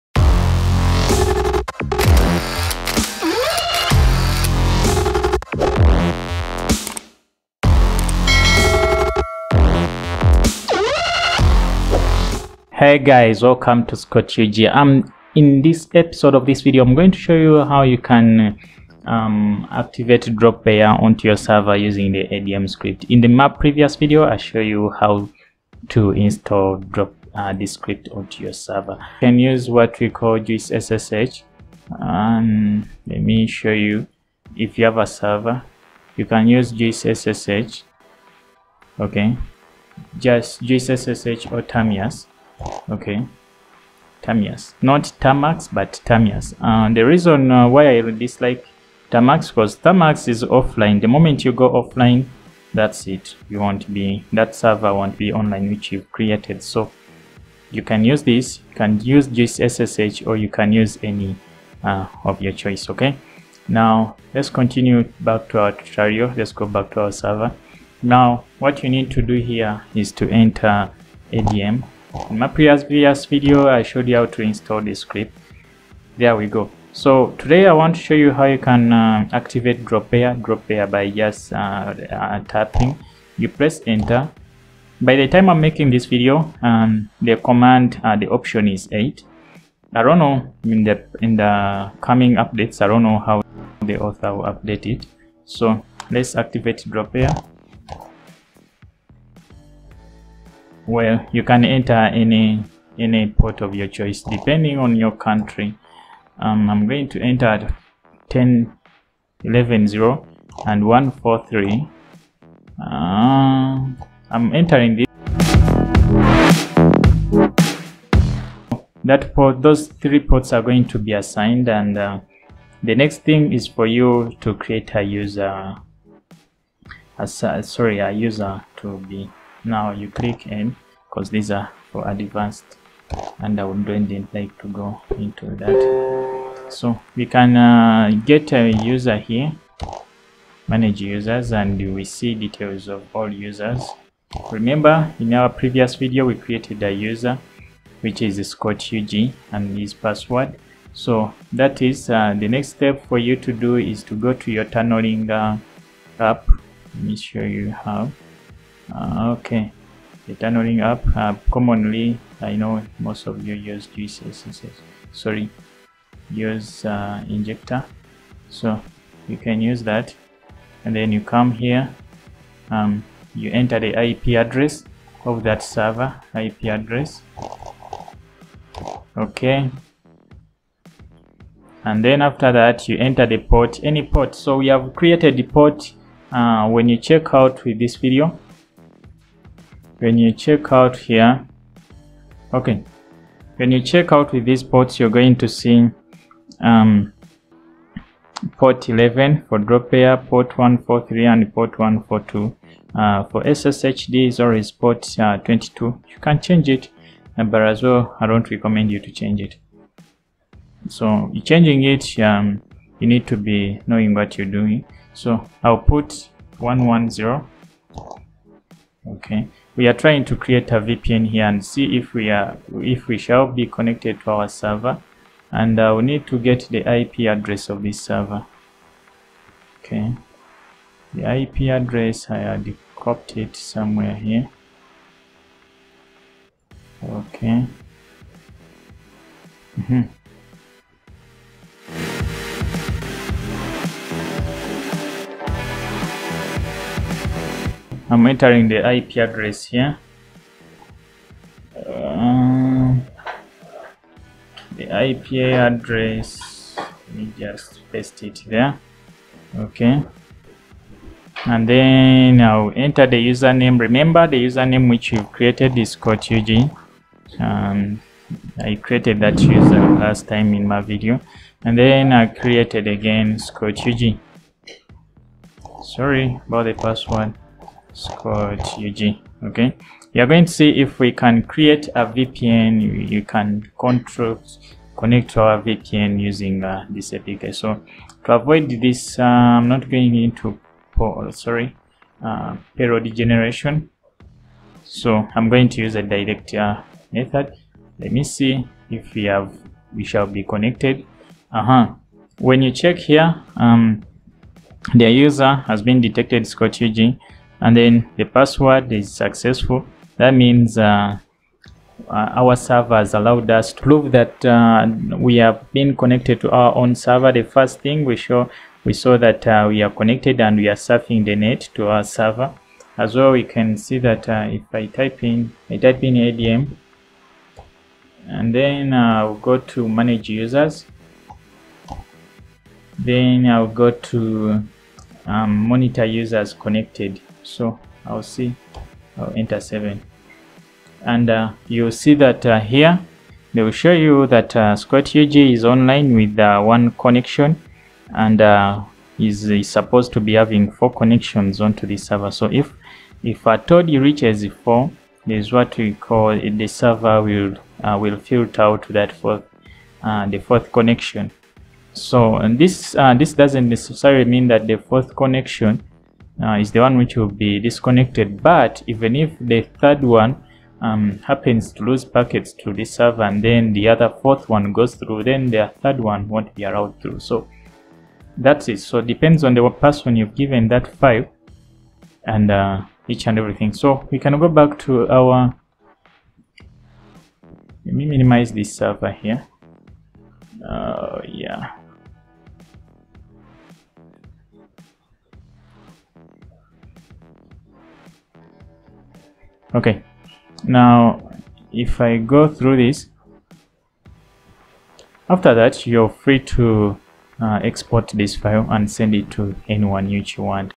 How To Open Dropbear Ports All Ubuntu Ports on VPS SSH, OpenVPN, TCP, UDP
How to open dropbear on server VPS Dropbear Ports on VPS SSH, OpenVPN, and other protocols is all we are to discuss in this video, dnt mind coding because  am going to use a one click installation script and will show you how to use it. enjoy!